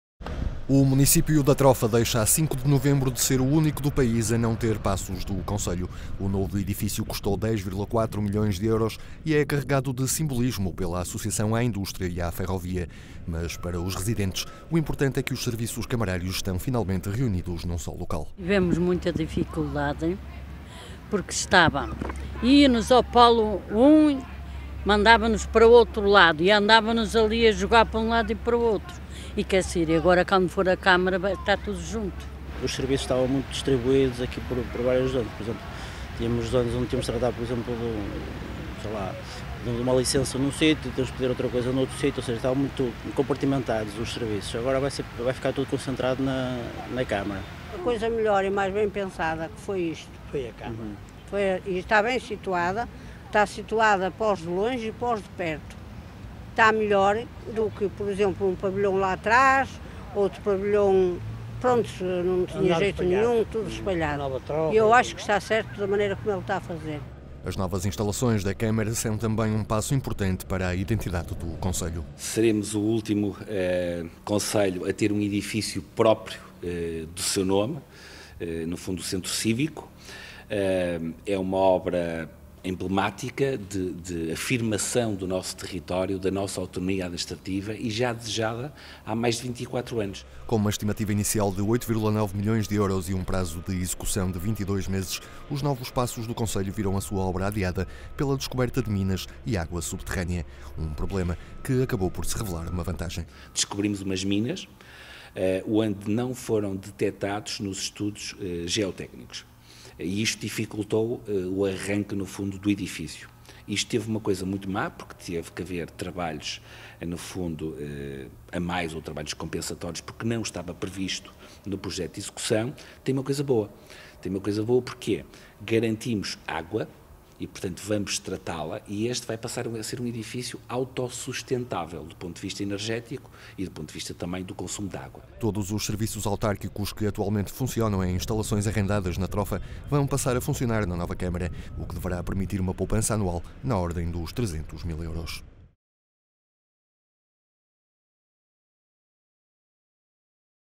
REPORTAGEM: Trofa deixa em novembro de ser o único município sem paços do concelho
O município da Trofa deixa a 05 de novembro de ser o único a não ter paços do concelho, inaugurando um edifício que custou 10,4 milhões de euros e carregado de simbolismo pela associação à indústria e à ferrovia.

Imagem e edição: André Sá